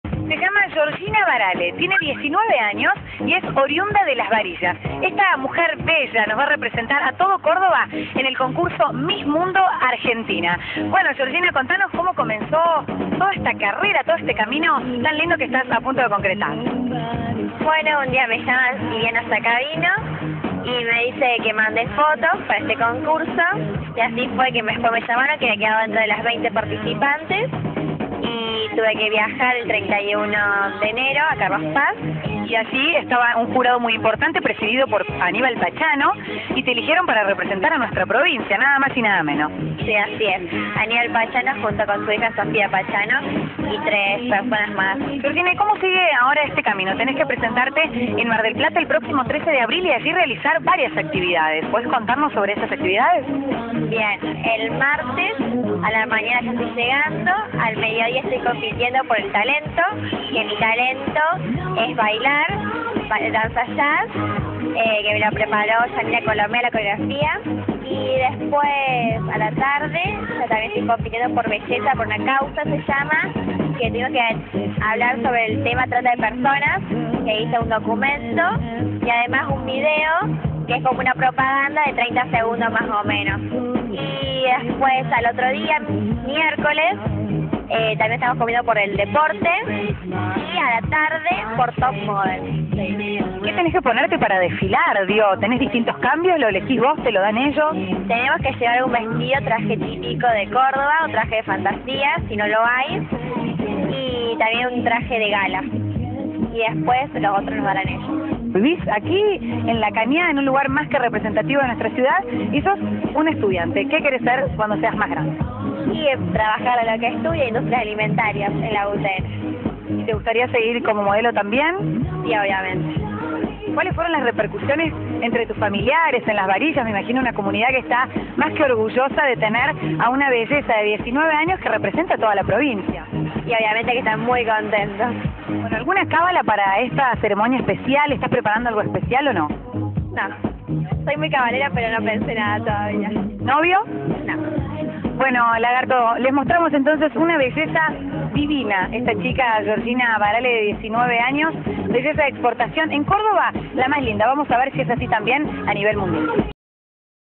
Georgina Barale representante cordobesa en el concurso Miss Mundo Argentina 20120405.3gp
Georgina Barale es la cordobesa que nos representará en el concurso Miss Mundo Argentina. Desde La Cañada, la modelo nos cuenta cómo será su participación ...